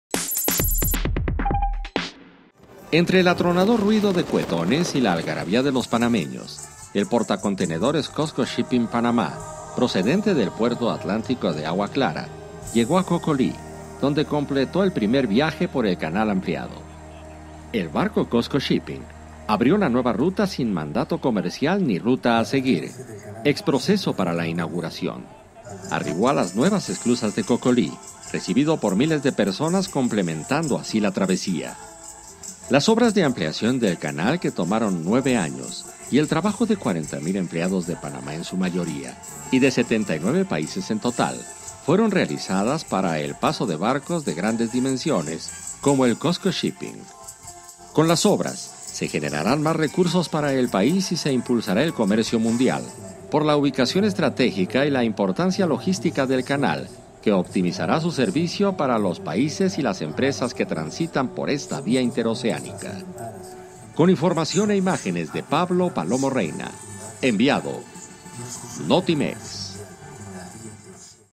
Histórica inauguración culmina en Canal Ampliado de Panamá
El portacontenedores Cosco Shipping Panamá, llegó a Cocolí, donde completó el primer viaje por el Canal Ampliado